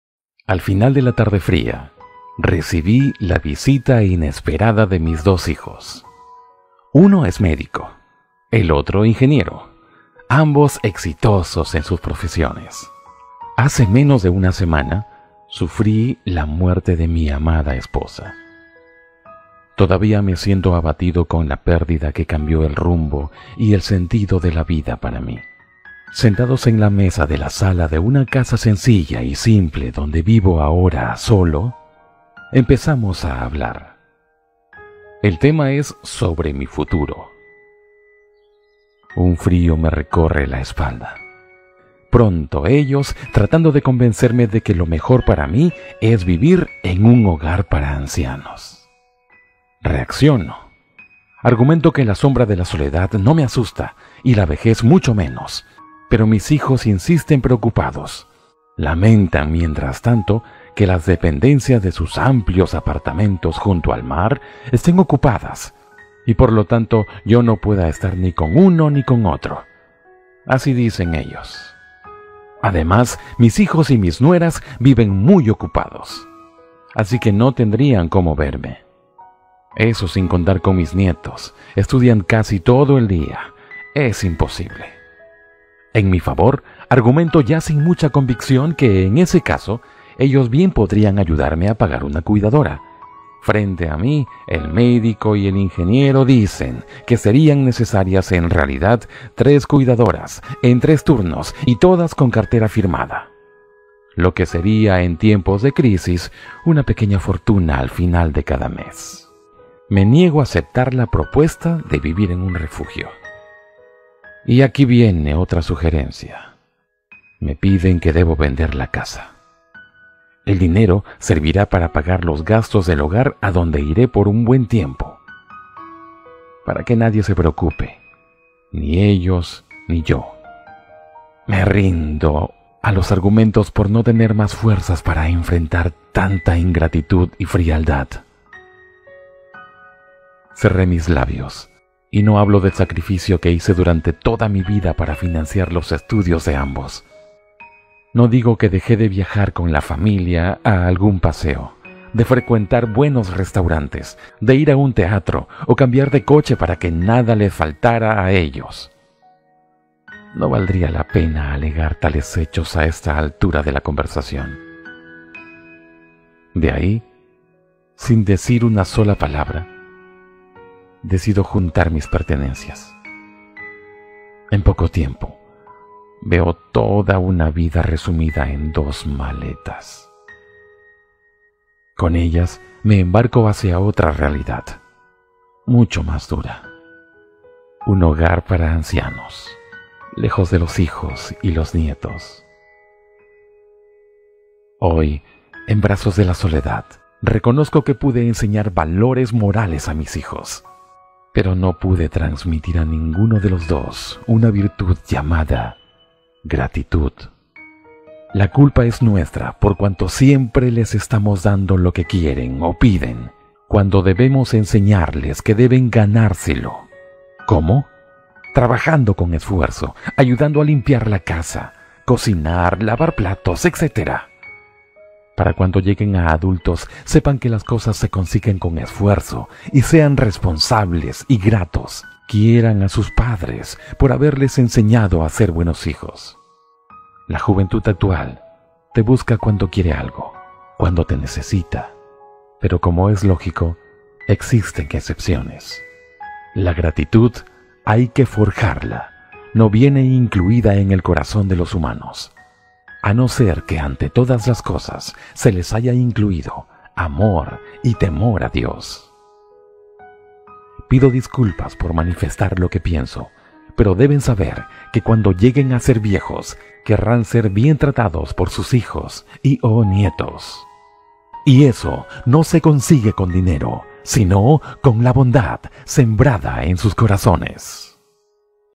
LOS HIJOS.
Video from OSCAR LOPEZ BAYER. Cuando el tiempo pasa y los padres llegamos a viejos que pasara?